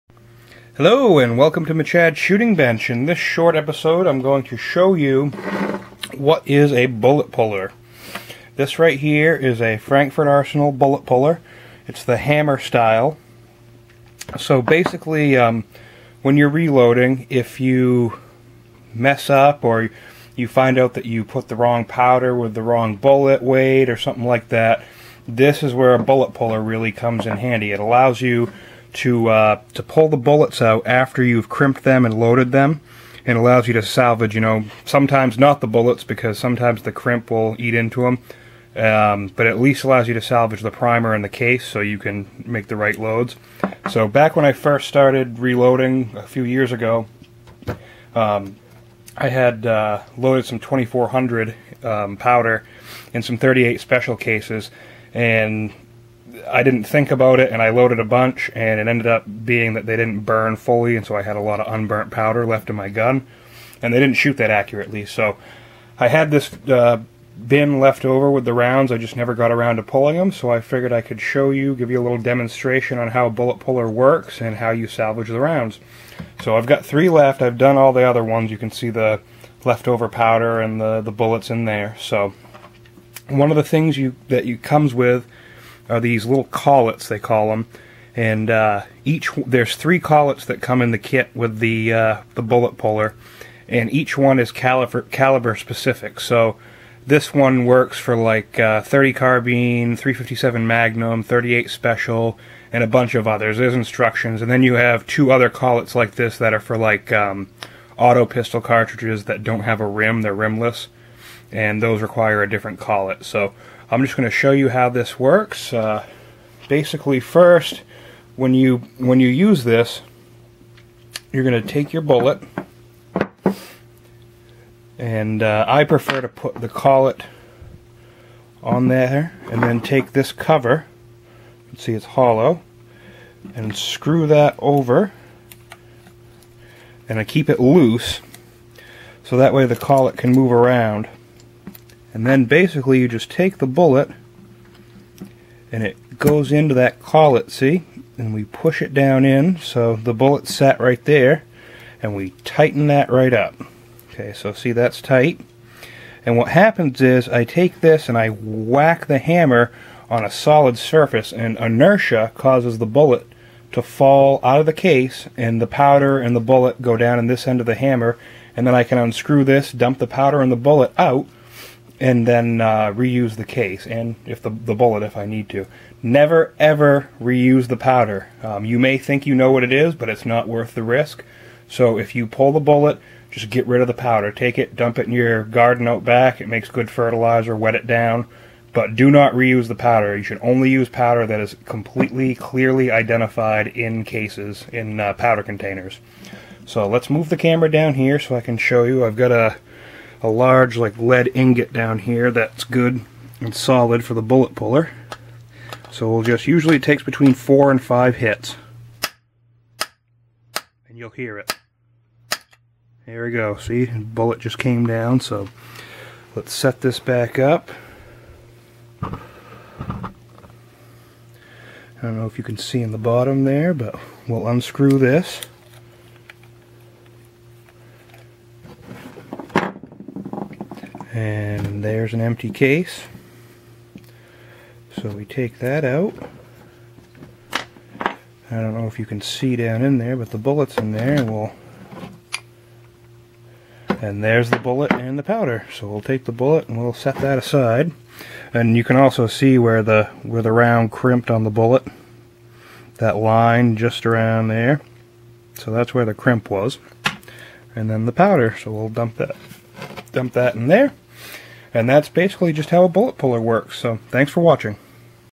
What Is a Bullet Puller & How Do You Use It?
This is a short video on what a bullet puller actually is and how you use a bullet puller.  There are two different types of bullet pullers, the featured one is a hammer-style bullet puller. There are also collet-style bullet pullers
for more firearm reviews!!